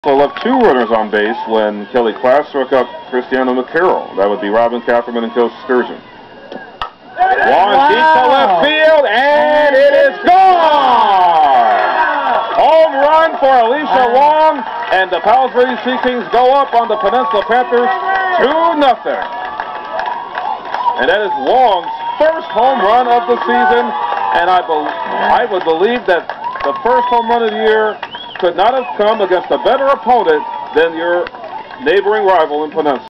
Alicia Wong's Two-Run Home Run!
With two outs in the bottom of the first inning, Palos Verdes High softball star Alicia Wong belts 0-and-2 pitch over the left field fence for a two-run home run, giving the Sea Kings a 2-0 lead over their rival Peninsula of Rolling Hills Estates. Palos Verdes eventually prevailed, 4-2 over the Panthers in this 2008 Bay League showdown.